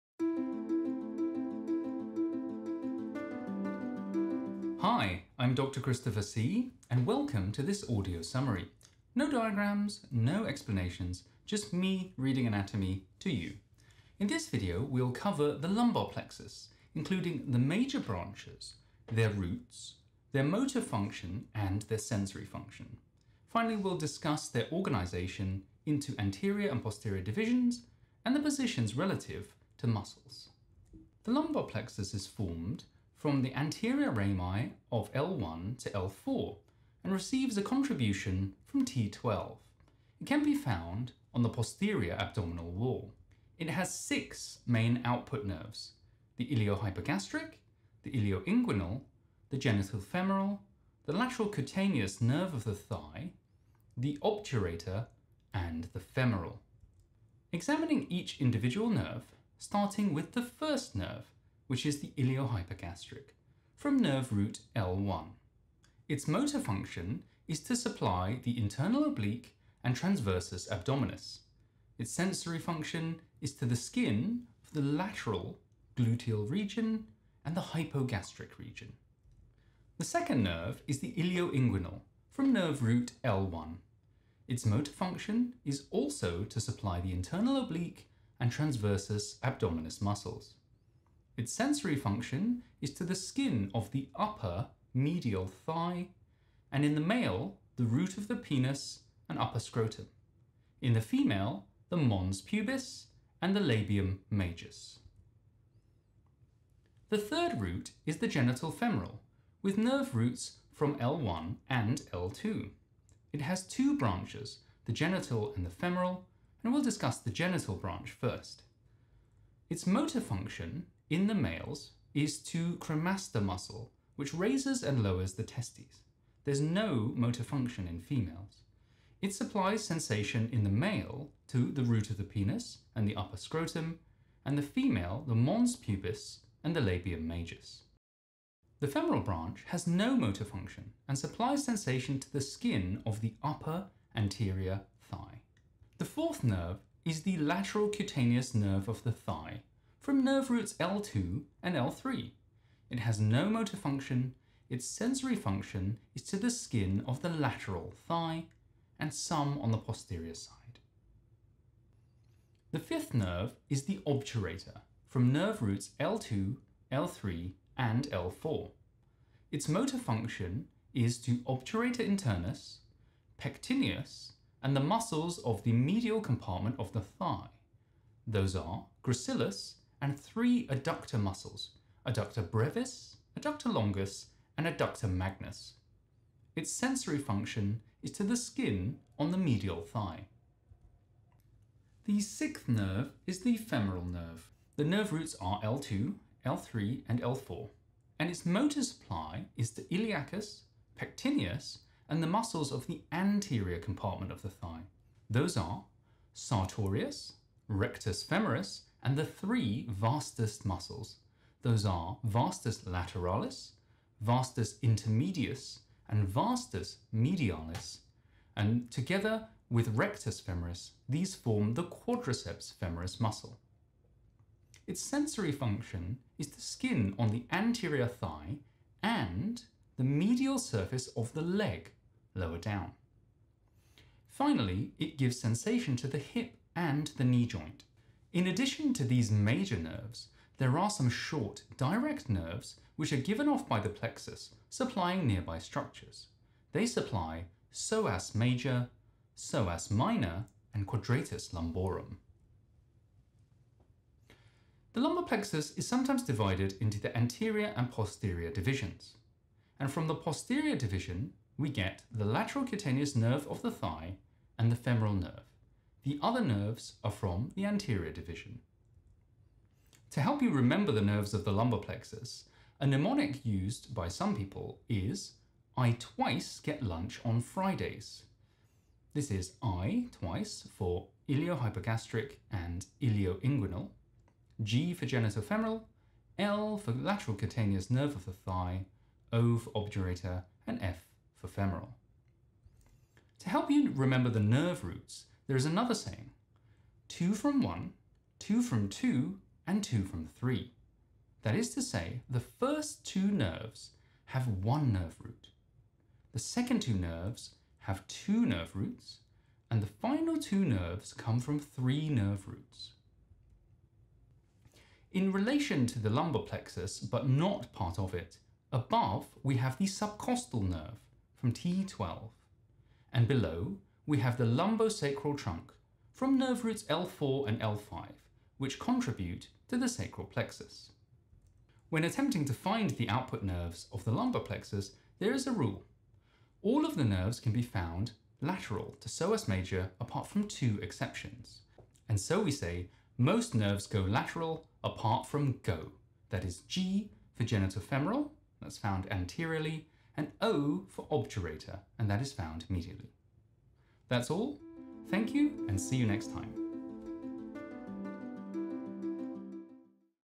Anatomy of the Lumbar Plexus - Audio summary by Dr. Christopher See
A summary of the 6 main nerves of the lumbar plexus, their nerve roots, motor and sensory function, and their organisation.